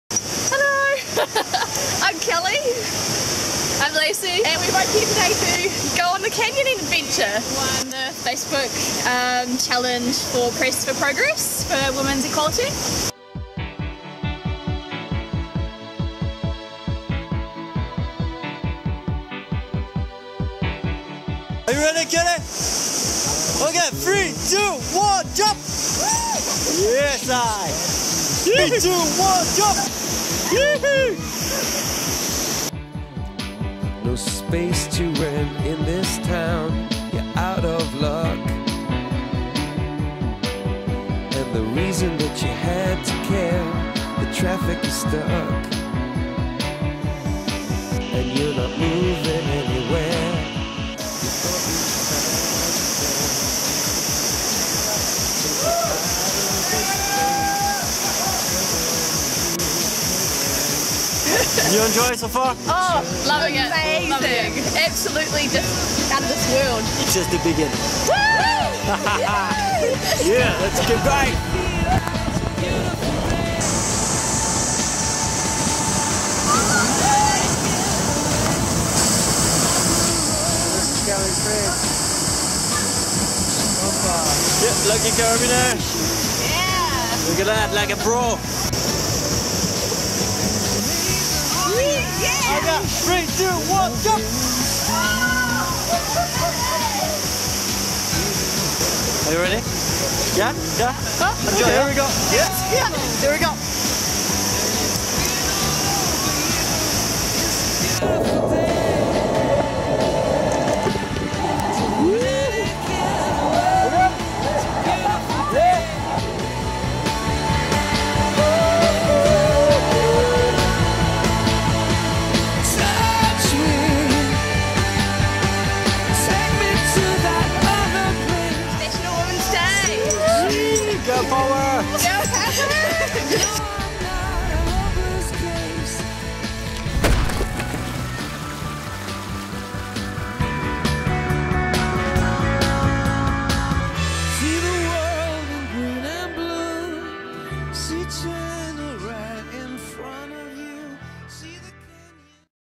INTERNATIONAL WOMEN'S DAY 2018
We took KELLY ROGERS and LACEY POPE, our INTERNATIONAL WOMEN'S DAY PressForProgress Facebook Competition WINNERS through the TORRENT RIVER. We did not see these two stop smiling and laughing all day. It was a joy to take you ladies with us on an adventure.